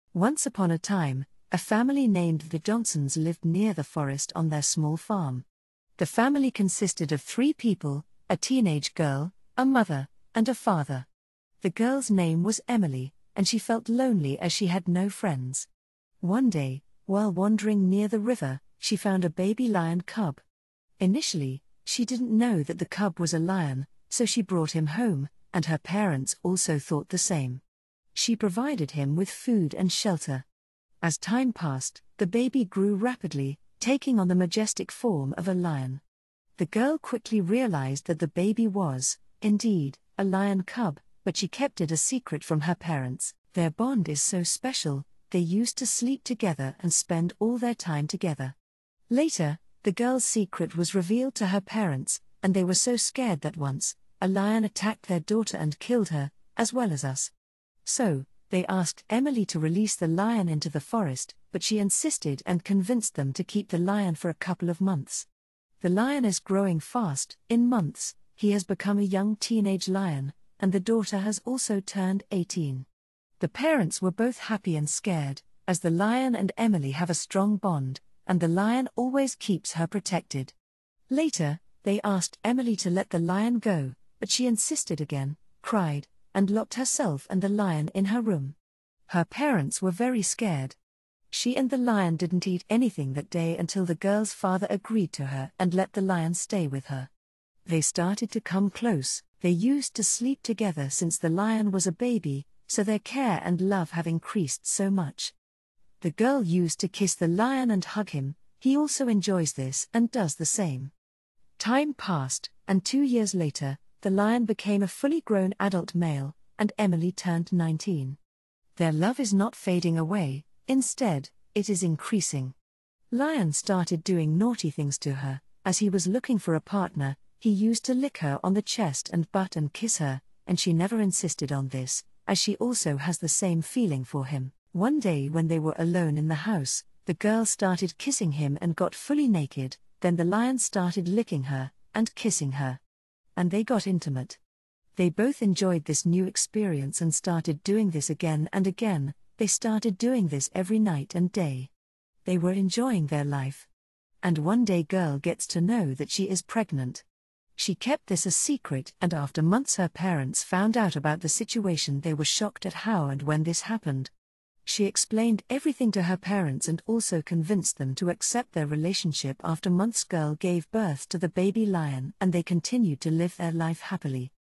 Girl Found A Baby Lion Fantasy Story
Today, we embark on a captivating journey into the realm of fantasy. Before we dive in, it's important to note that all characters, events, and places discussed in this video are entirely fictional. This video is intended for audiences aged 16 and above.